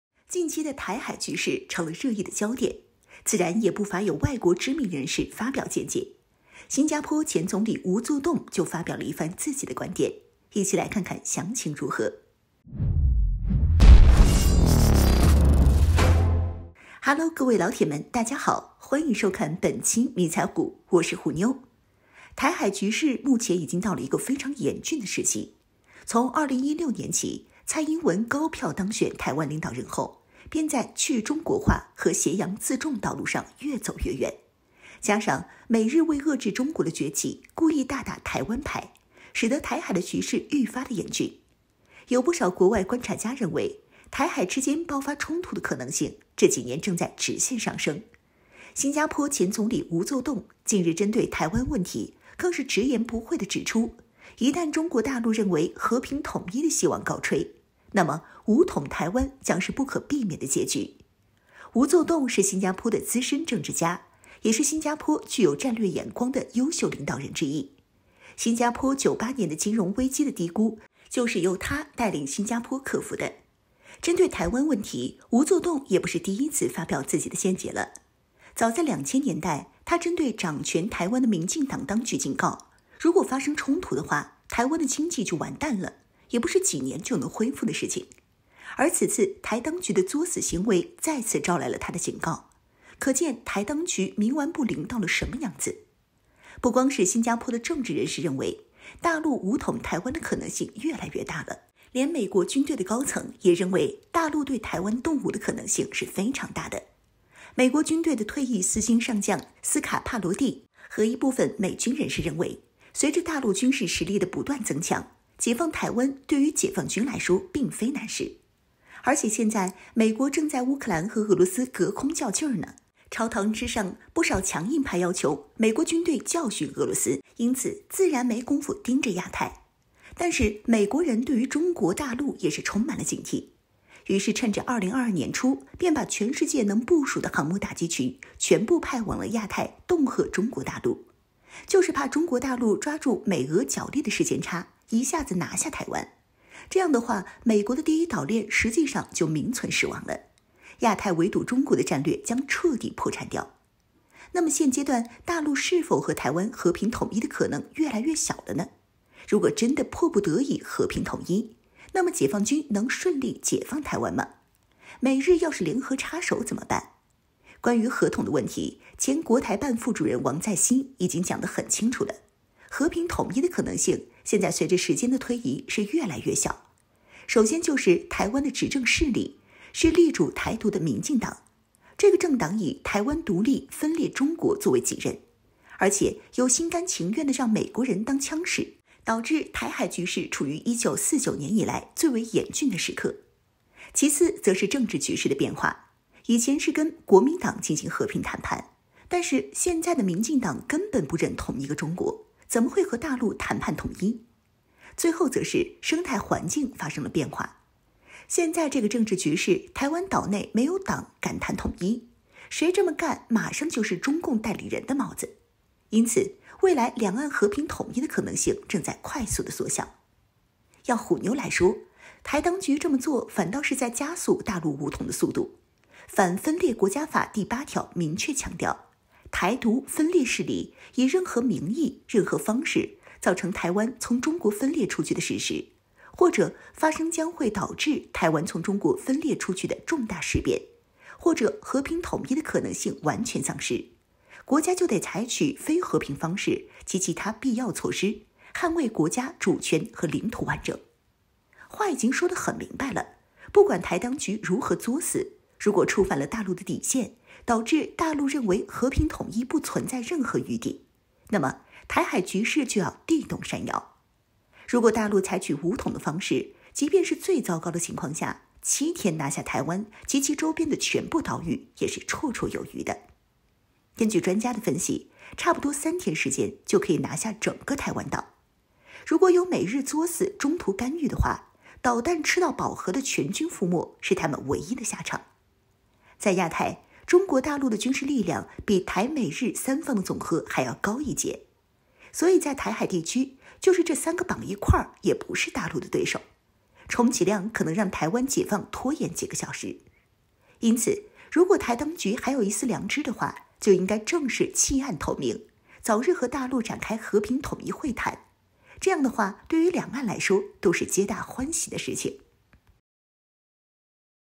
新加坡前总理：若大陆和台湾无和统可能，武统台湾是唯一选项
军事 热门军事 军事解说 我們的影片會遵循公平、尊重的原則，確保不會包含任何仇恨言論的元素。